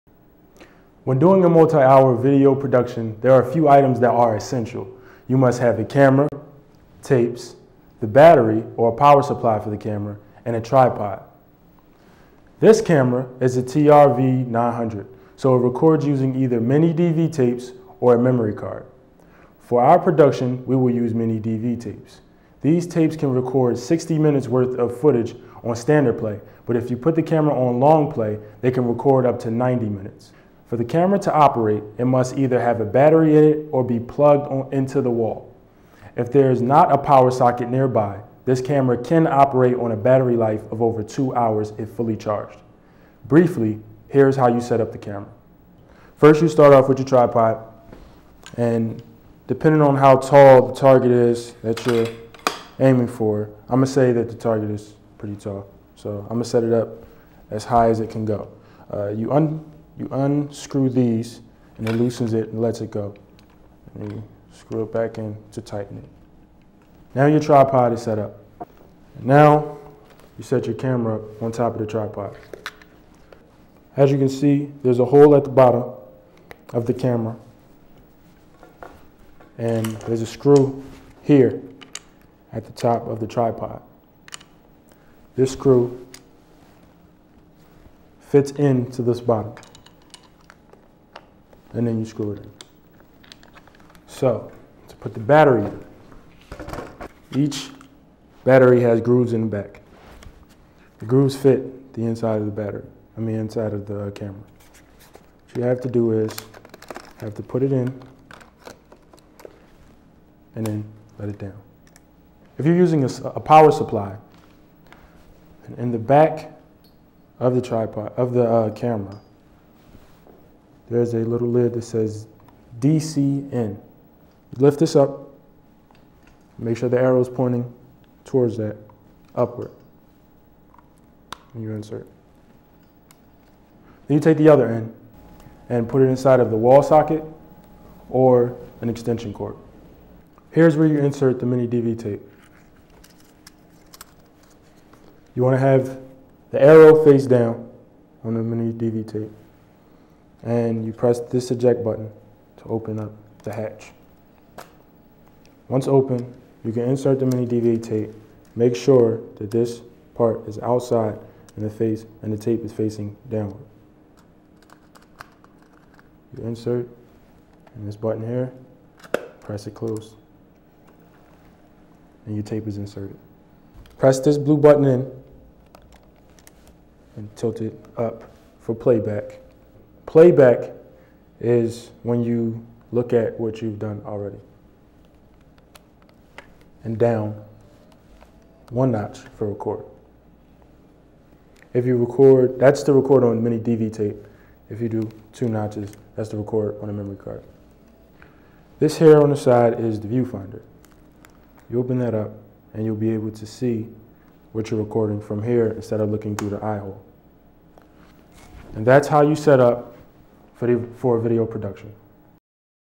How to Set Up a Single Camera Video Production 2009
This was a video that I used in a presentation for one of my classes at West Chester University.